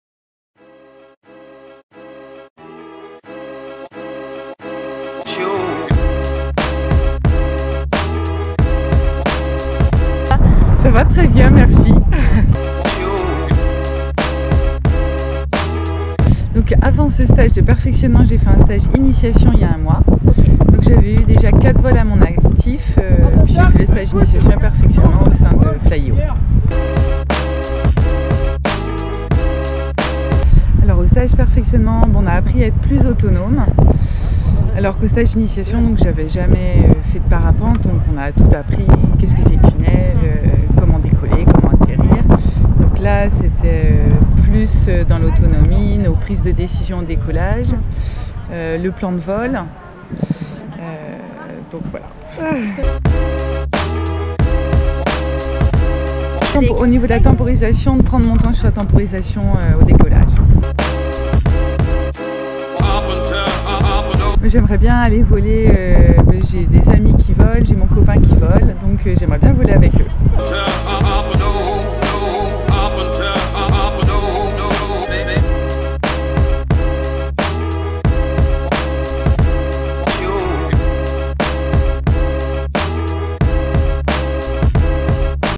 Interview avec Céline en stage perfectionnement parapente à Annecy
Rencontrez Céline lors de son dernier jour en stage perfectionnement. Tous les pilotes sont les bienvenus pour se perfectionner avec notre équipe de moniteurs :)

Images et montage: Lorenza Bertaud